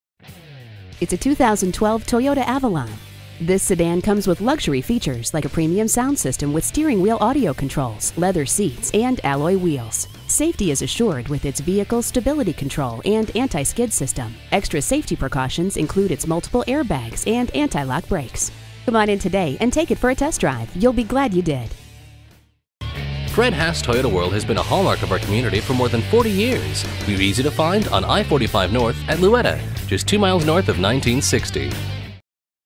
Used 2012 Toyota Avalon Spring TX, TX CU464350T - SOLD
Year: 2012
Make: Toyota
Model: Avalon
Trim: Limited
Engine: 6 Cyl. 3.5L
Transmission: Automatic
Color: Maroon
Mileage: 67643
Stock #: CU464350T
VIN: 4T1BK3DB9CU464350
Delivers 28 Highway MPG and 19 City MPG! Carfax One-Owner Vehicle. This Toyota Avalon boasts a Gas V6 3.5L/211 engine powering this Automatic transmission. XM satellite radio -inc: 90-day trial subscription, Woodgrain style interior trim, Woodgrain & leather-wrapped steering wheel.*This Toyota Avalon Features the Following Options *Woodgrain & leather-wrapped shift knob, Vehicle stability control (VSC) w/off switch, Variable-assist pwr rack & pinion steering, Traction control (TRAC), Tilt/telescoping steering wheel w/audio & climate controls, Smart Key system -inc: push-button start, panic alarm, trunk release, remote illuminated entry w/puddle lamps, Side-impact door beams, Rear passenger reading lamps, Rear flat floor, Rain-sensing washer-linked variable intermittent windshield wipers.*Know You're Making a Reliable Purchase *According to Carfax's history report: Carfax One-Owner Vehicle, No Damage Reported, No Accidents Reported, 10 Service Records.*Stop By Today *A short visit to Fred Haas Toyota World located at 20400 Interstate 45, Spring, TX 77373 can get you a trustworthy Avalon today!

Address:
20400 Interstate 45 North